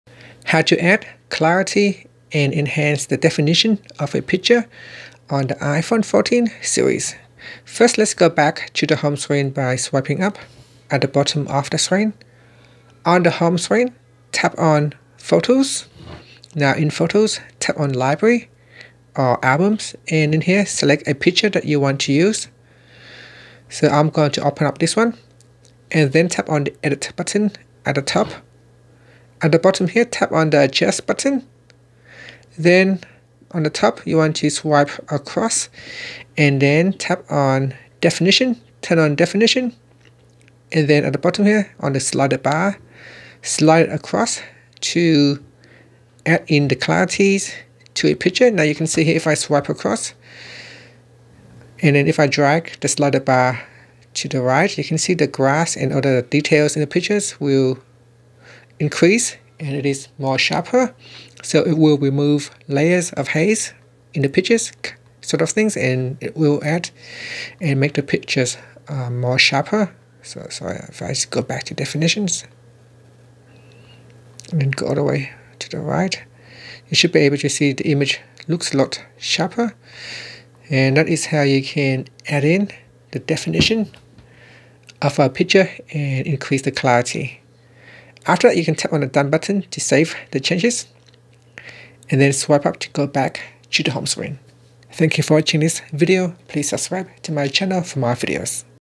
iPhone 14's/14 Pro Max: How to Add Clarity and Enhance the Definition of a Picture
Learn how you can add clarity and enhance the definition of a picture on the iPhone 14/14 Pro/14 Pro Max/Plus.